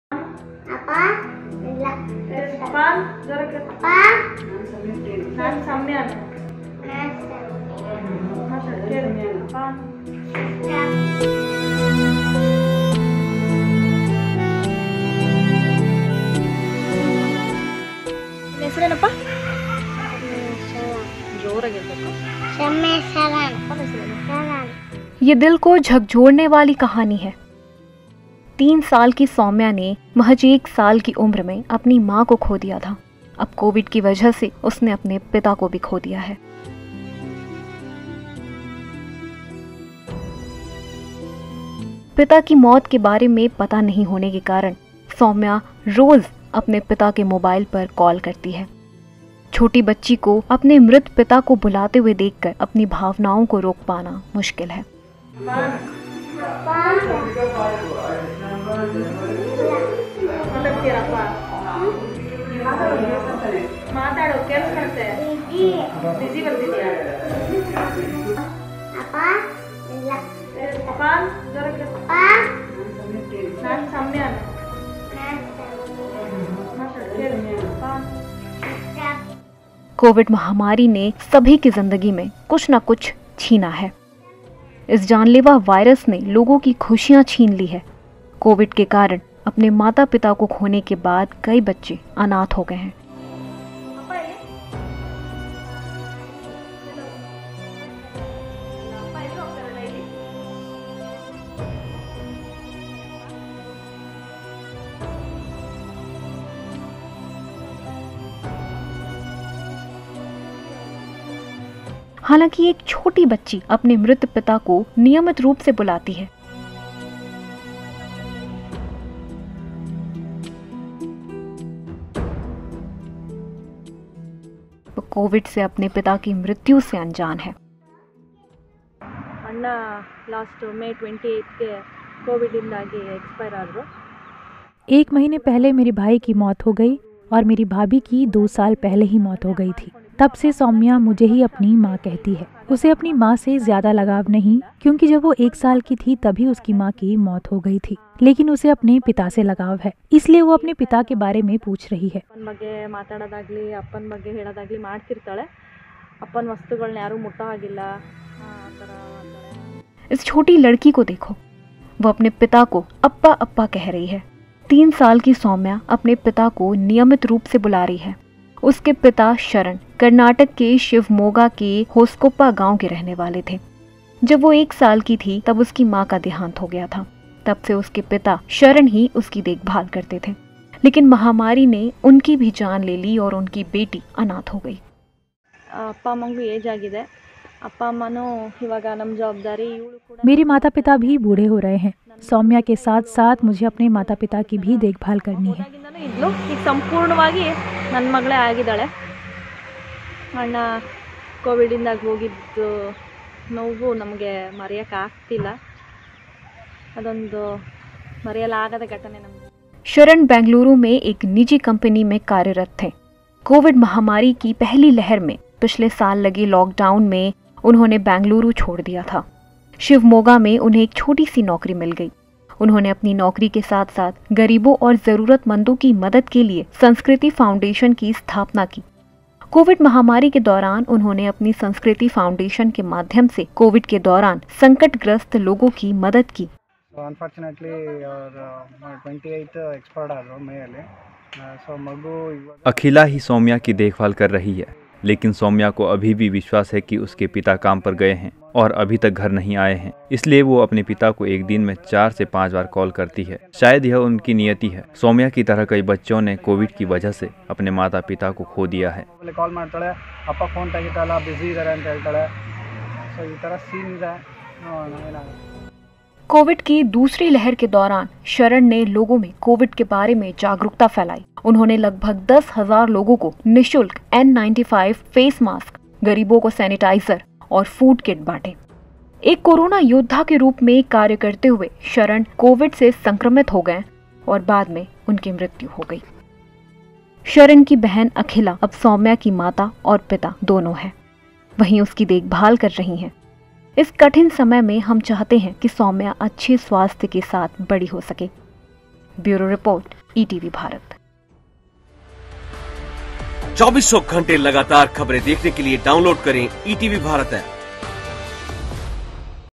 अप्पा... तुम कब आओगे? | Little Sowmya calling her deceased father | ETV Bharat
कोरोना महामारी ने सभी की जिंदगी से कुछ न कुछ छीना है. इस जानलेवा वायरस ने किसी की खुशियां छीन ली, तो कई बच्चे अनाथ हो गए हैं. ऐसी ही एक दिल को झकझोर देने वाली कहानी है महज 3 तीन साल की सौम्या की. कर्णाटक के शिवमोगा में रहने वाली सौम्या ने महज एक साल की उम्र में अपनी मां को खो दिया था. अब कोविड की वजह से उसने अपने पिता को खो दिया है. पिता की मृत्यु के बारे में पता नहीं होने के कारण सौम्या नियमित रूप से अपने पिता के मोबाइल पर कॉल करती है, और उन्हें बुलाती है अप्पा तुम कब आओगे? देखिये ये खास रिपोर्ट